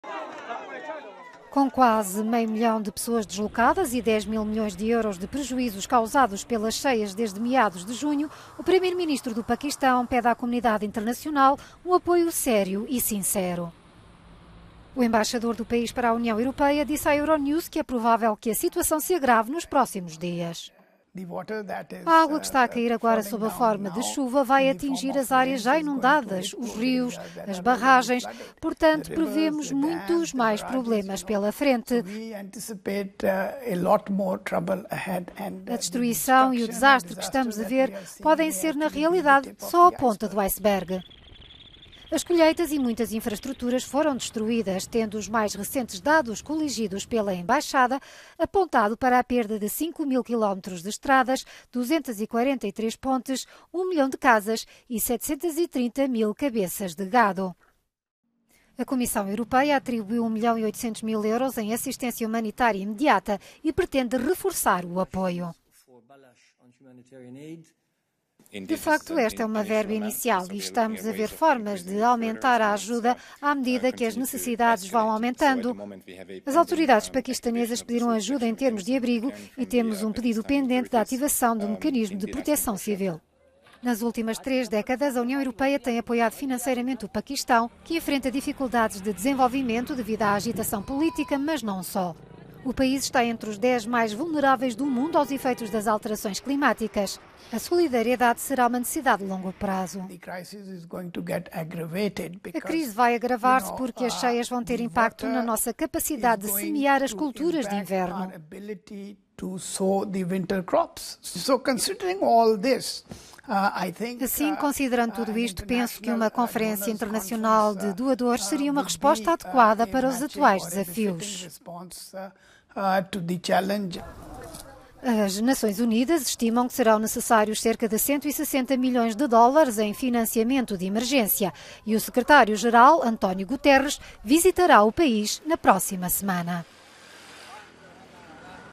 Embaixador do Paquistão pede mais ajuda à UE para enfrentar as cheias
Embaixador do Paquistão pede mais ajuda à UE para enfrentar as cheias. Asad Majeed Khan, disse à euronews que é provável que a situação se agrave nos próximos dias.